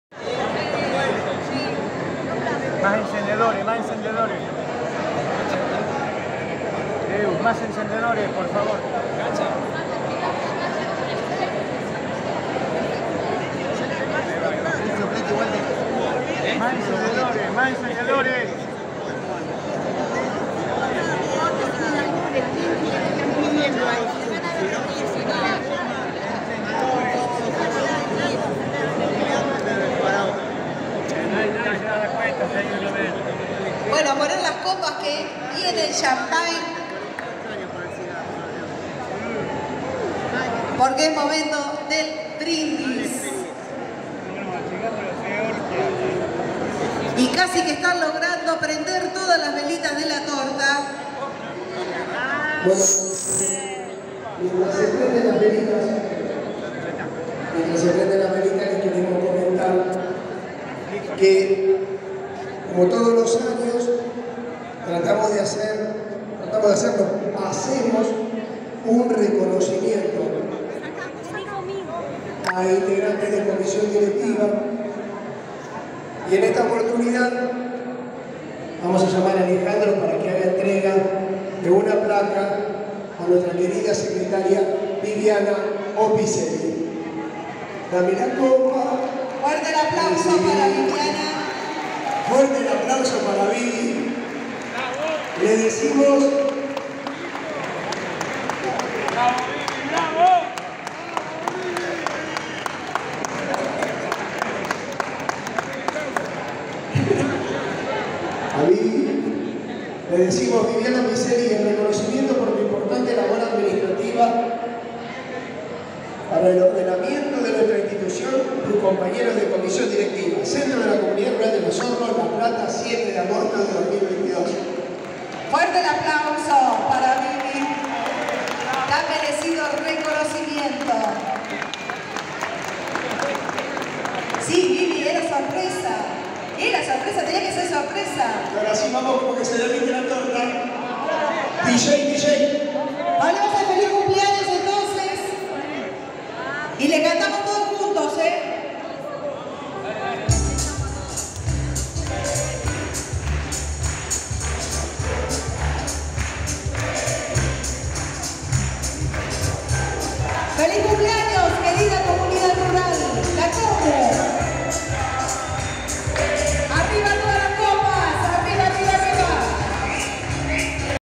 Aniversario de Comunidad Rural
La Comunidad Rural de Los Hornos celebró sus 60 años de vida